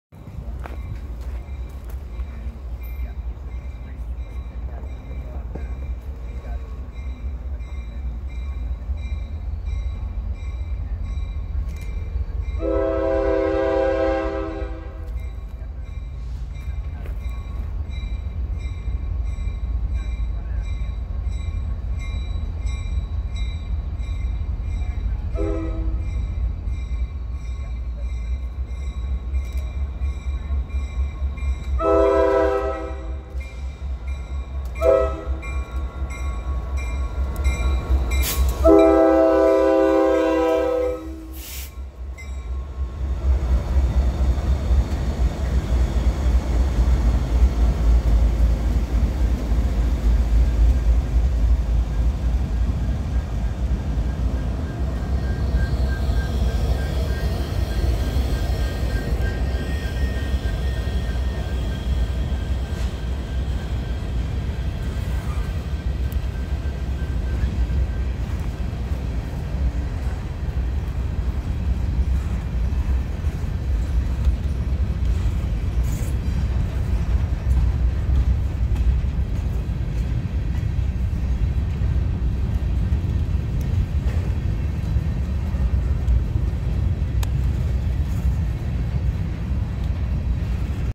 NS 7148 plus 3 GEs and another GP60 leads unit grain empty east through Porter, IN
I've been longing for one of these grain trains, so when the opportunity presented itself as I was about to head home, I headed a mile across town to obtain this uncommon move.
Only ITCS equipped motors can lead on the Amtrak Michigan Line, therefore GP60s are the only motors NS has that are capable of leading on it.
NS 7148, NS 9737, NS 7684, NS 4537, NS 7145
7-18-22